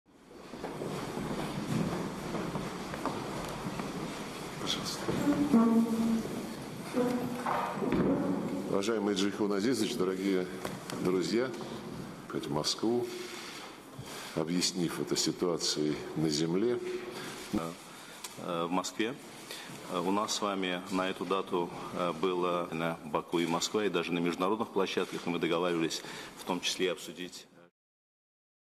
Russian Foreign Minister Lavrov hosts Azerbaijani counterpart Bayramov in Moscow | AFP
Images of Russian Foreign Minister Sergei Lavrov hosting his Azerbaijani counterpart Jeyhun Bayramov in Moscow. On December 22, Aremania accused Russian peacekeepers of failing to prevent an "illegal blockade" in Azerbaijan's Armenian-populated Nagorno-Karabakh region which has sparked two wars between the neighbouring nations.